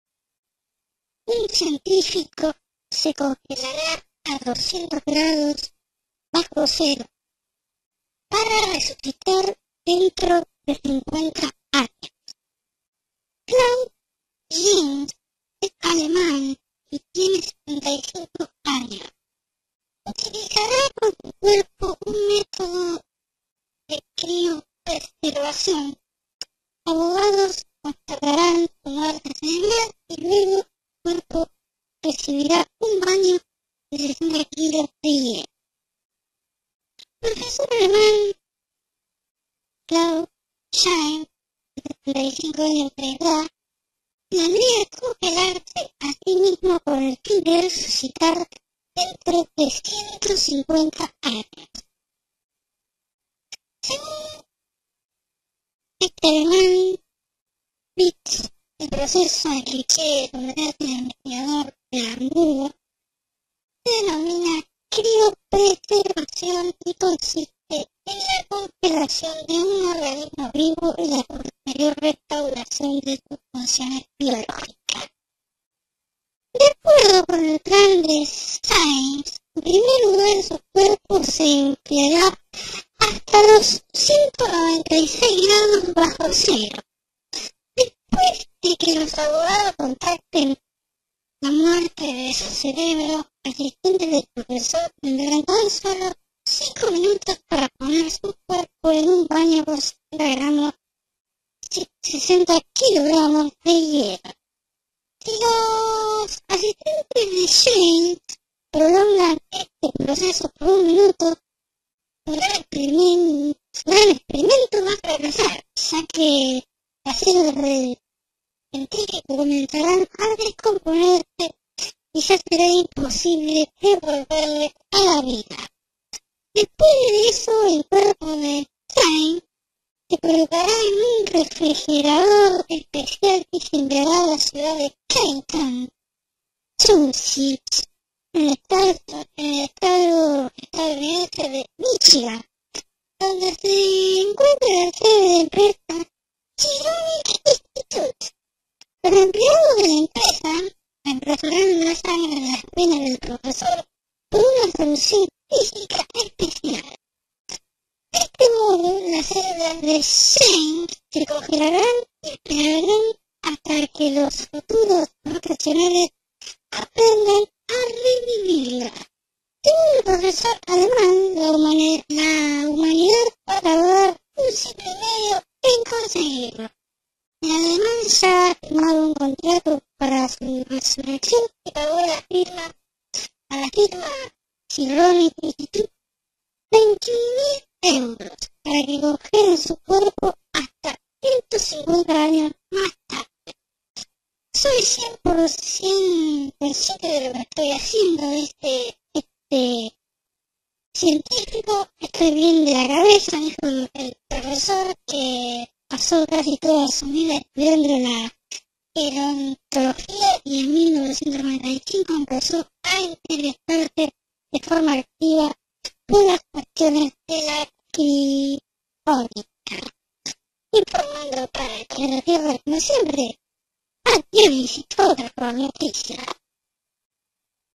150 años se congelara un profesor
Un profesor alemán se congelara durante 150 años y espera ser revivido luego ,cuando la medicina logre un gran adelanto para resucitar a los congelados .